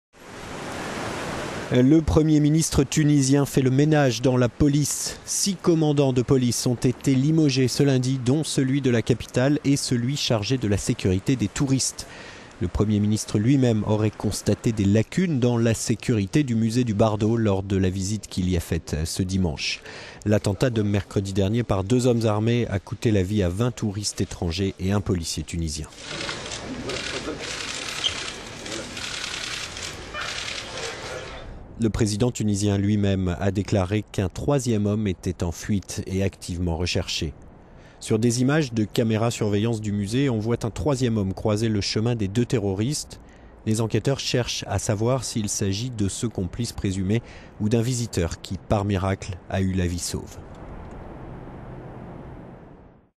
Attentat du Bardo en Tunisie : six commandants de police limogés
Le premier ministre tunisien fait le ménage dans la police. Six commandants de police ont été limogés ce lundi, dont celui de la capitale et celui chargé de la sécurité des touristes.

Le Premier ministre lui-même a dit avoir constaté des lacunes dans la sécurité du musée du Bardo lors de la visite qu'il y a faite dimanche.

L'attentat de mercredi dernier par deux hommes armés a coûté la vie à vingt touristes étrangers et un policier tunisien.